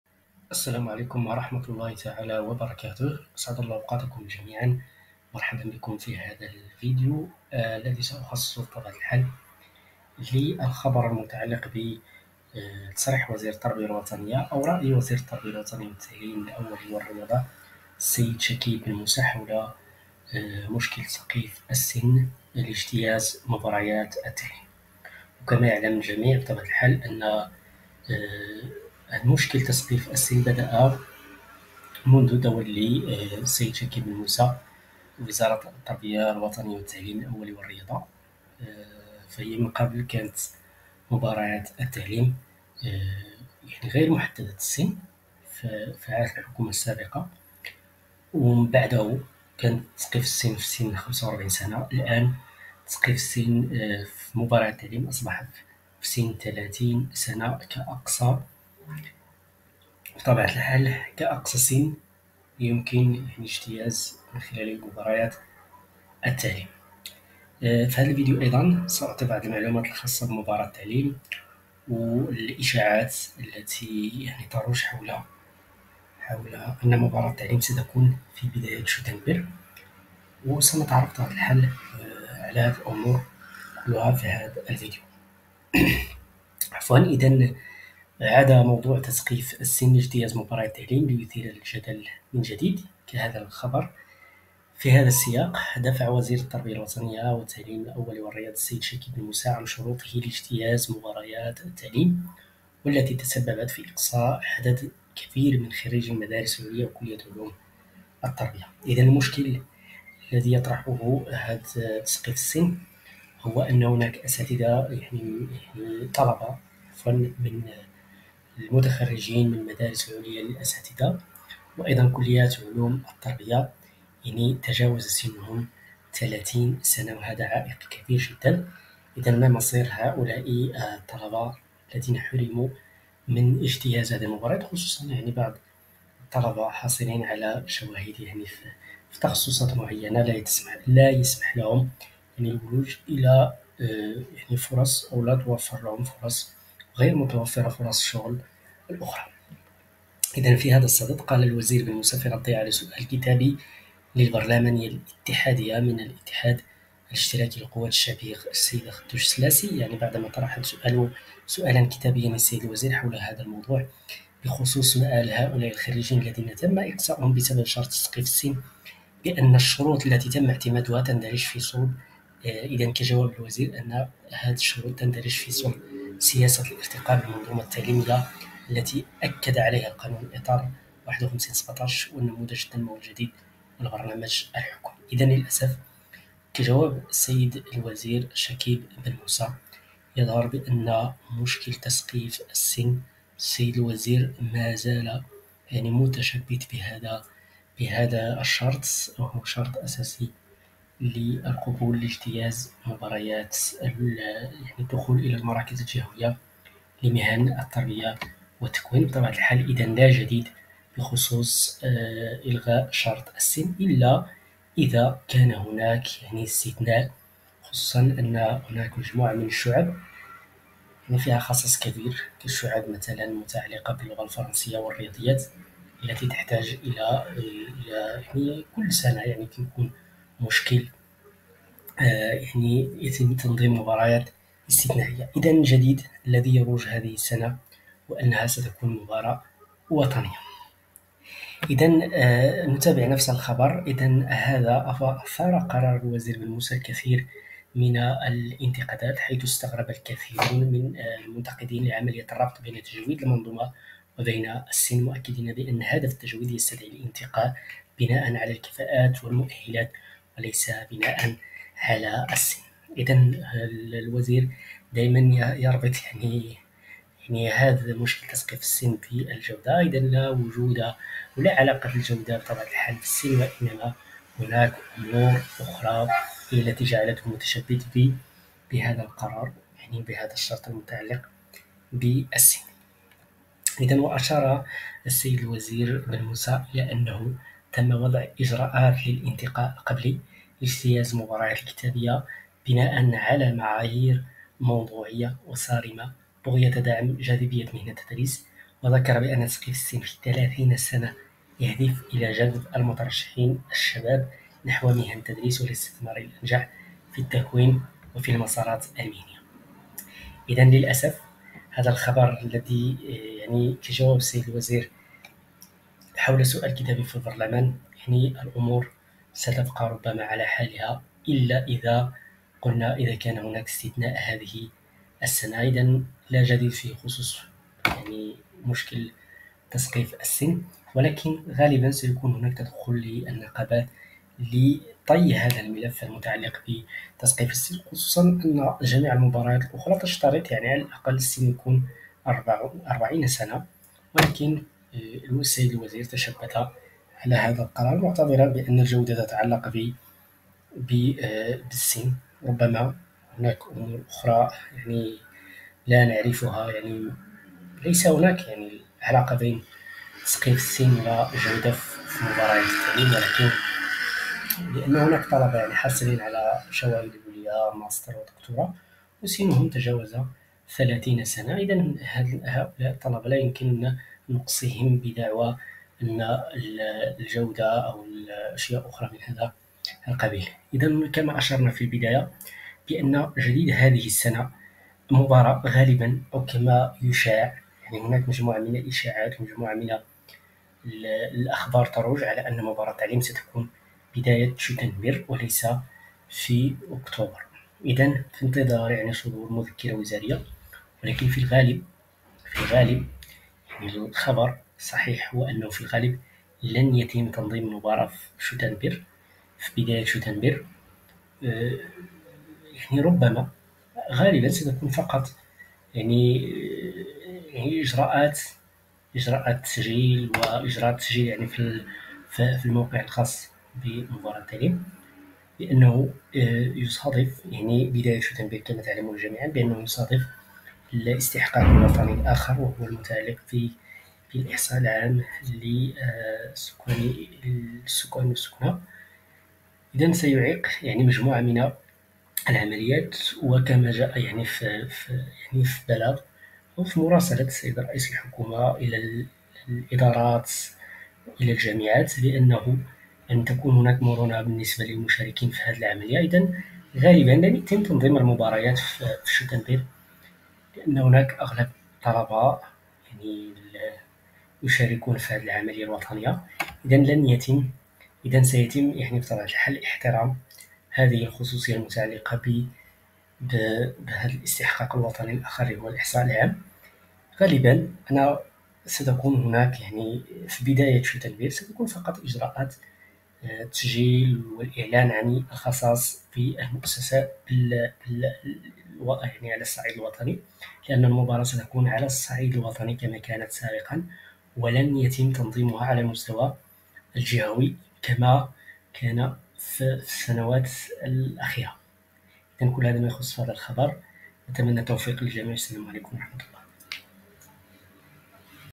وزير التربية الوطنية و تسقيف السن لاجتياز مباريات التعليم، هذه أهم مستجدات مباراة التعليم 2024
في هذا الفيديو ستجدون أهم المستجدات التي تخص مباراة التعليم 2024
تسقيف السن
إلغاء شرط السن
مباراة التعليم 224
جديد مباراة التعليم 2024